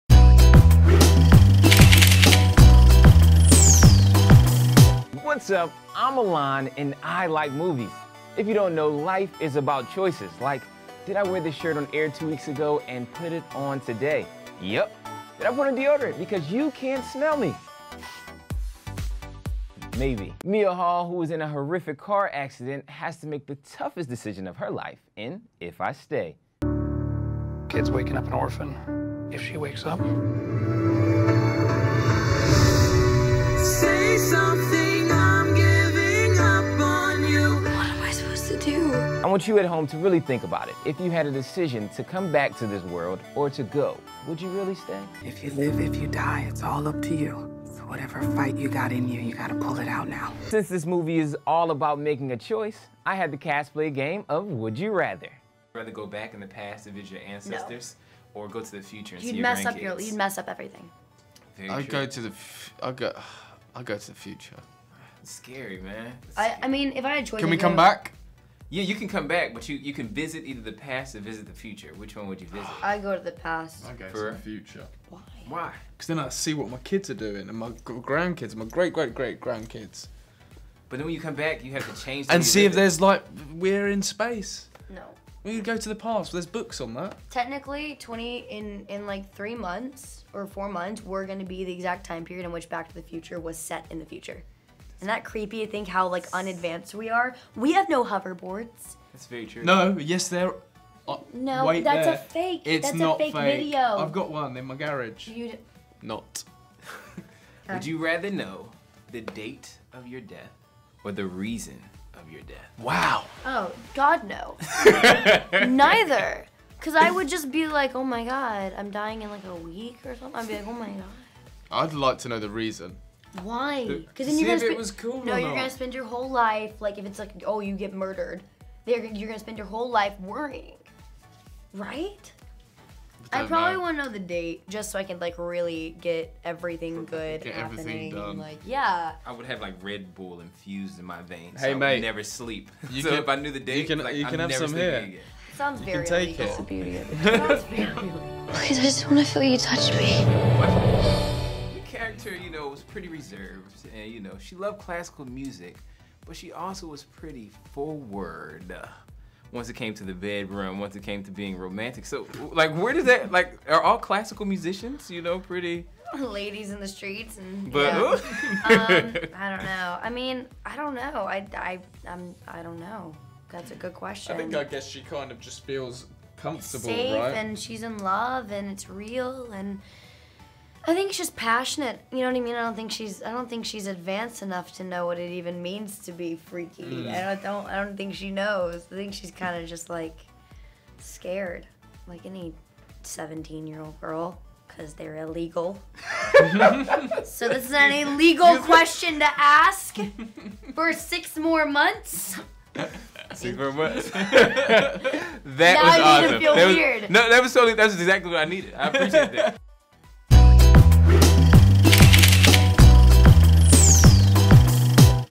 Chloë Grace Moretz and Jamie Blackley play 'Would You Rather' - Movies With Milan
This week Milan chats to Chloë Grace Moretz and Jamie Blackley, stars of the new movie 'If I Stay' and plays an hilarious game of Would You Rather.